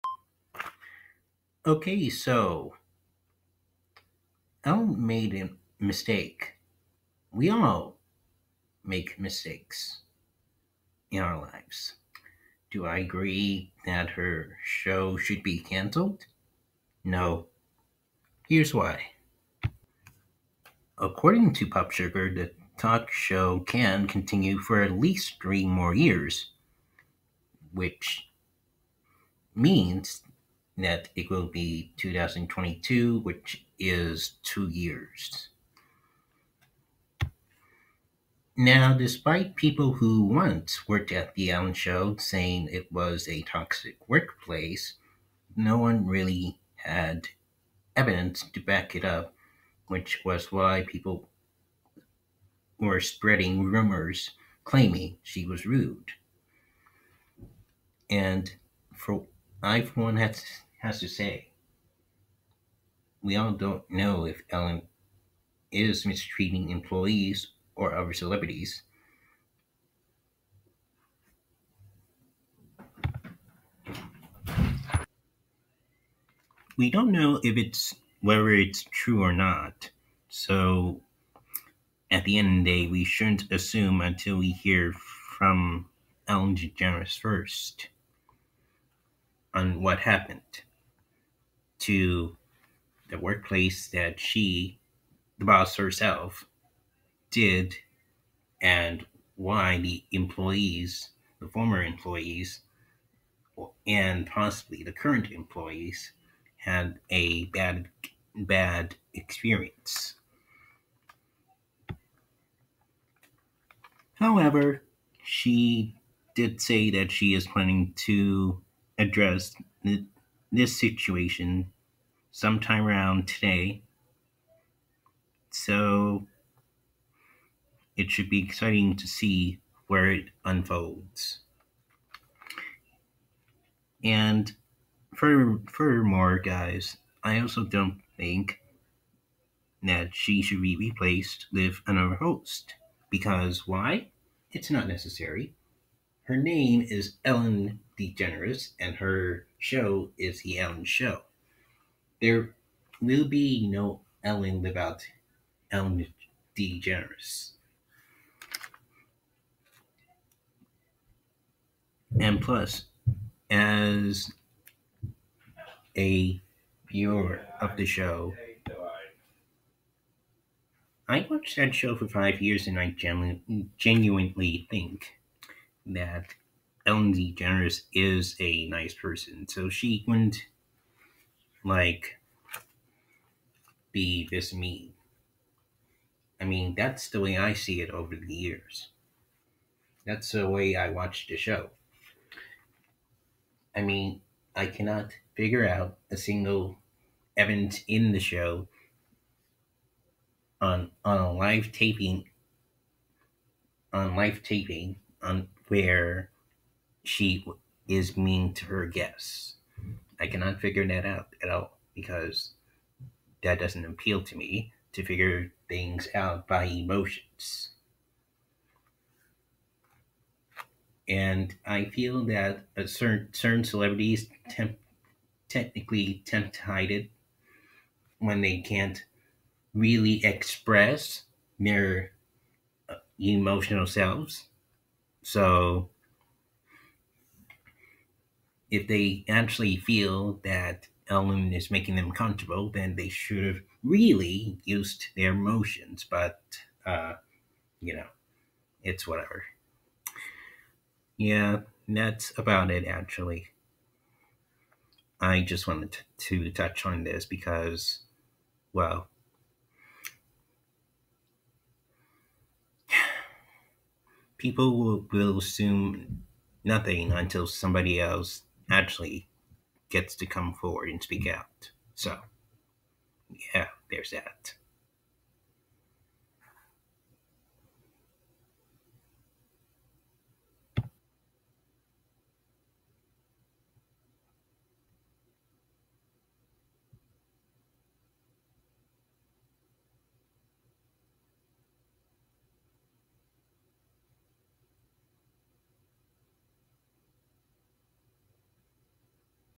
Defending Ellen DeGeneres | RTWRP #128
Ellen DeGeneres hasn't been getting the backlash that much when rumors about Ellen came up since 2015, it's only until NOW when the former employees came out during the coronavirus quarantine situation since the country was shut down in March 2020 when it all unfold in April. Later on more of Ellen's former employees came out in June & July even August.

People have accused Ellen DeGeneres for being mean or rude to some of her guest once.
Some also say that Ellen was rude or mean to her staff for years.

 However, as WarnerMedia has been investigating there is no evidence (in my opinion) of a leaked footage showing Ellen DeGeneres being the one who mistreated her employees for years. So we ALL shouldn't jump to conclusions & until we REALLY know, Ellen will address the situation herself. The Ellen DeGeneres Show should premiere some time today so it should be interesting to see.